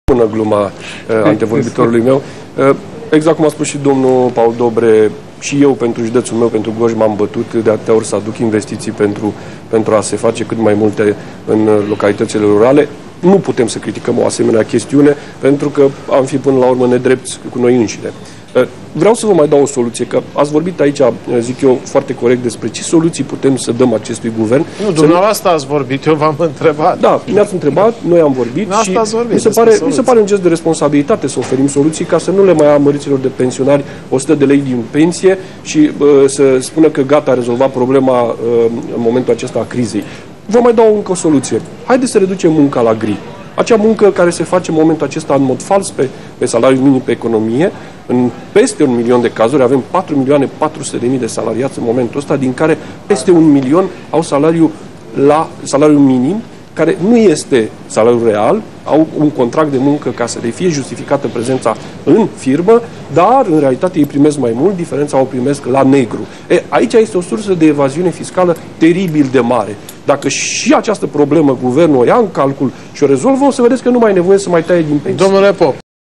Surupaceanu despre investitiile Guvernului Boc
Deputatul PSD Mugurel Surupaceanu despre sumele alocate de Guvernul Boc localitatii Rachitele (847 locuitori) pentru construirea unei sali de sport (2,4 mil lei)